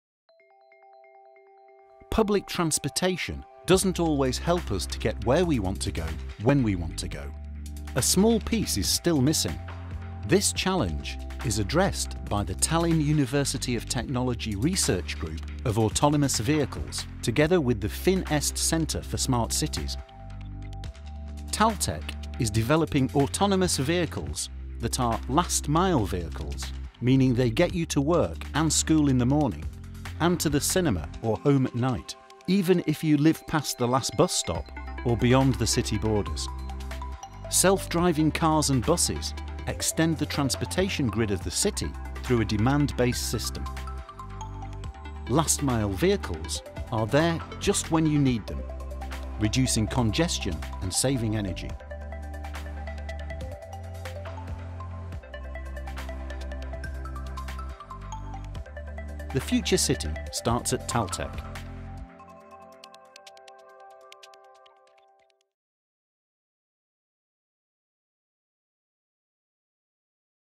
The transport of the future city - Dubai Expo TalTech
Public transportation doesn’t always help us get to where we want to go when we want to go – a small piece is still missing. This challenge is addressed by the Tallinn University of Technology research group of autonomous vehicles, together with the FinEst Centre for Smart Cities.

TalTech iseAuto - the AV shuttle project of Autonomous Vehicle research group